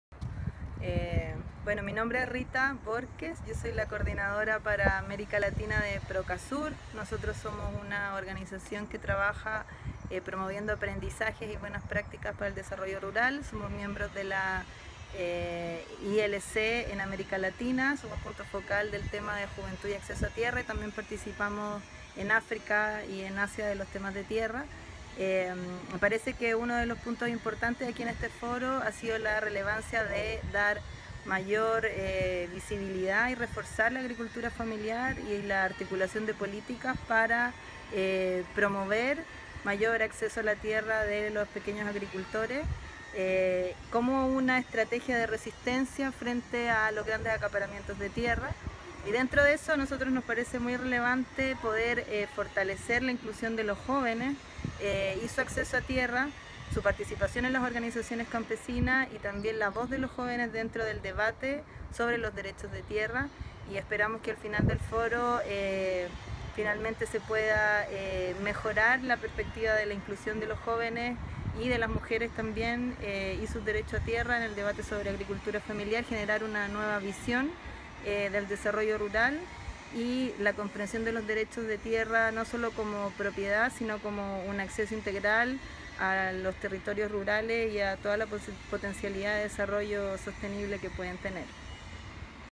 WFAL 2016 - Rita Bórquez (Procasur)
Rita Bórquez (Procasur) comparte los avances de la iniciativa Juventud Rural en ALC en el Foro Mundial Sobre El Acceso a la Tierra 2016 (Valencia, España).